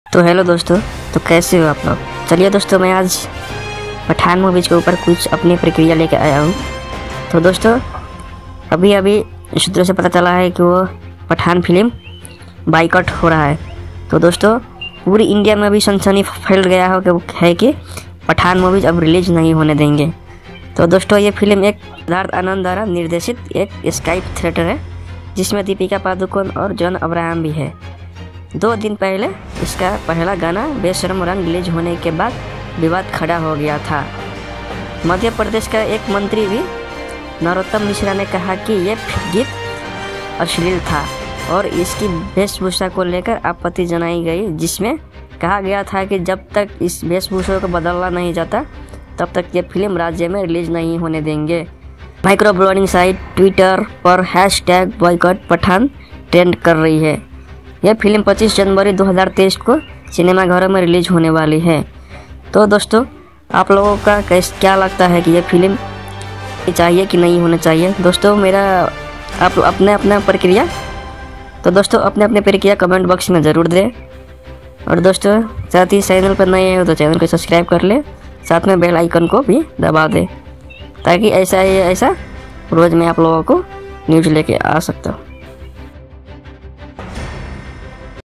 boycot सुशांत के फैंस ने शुरू किया बायकॉट पठान का ट्रेंड ।। कहा आमिर का घमंड तोड़ा अब शाहरुख की बारी
Pathan Pathan Public Review PathanPublic Reaction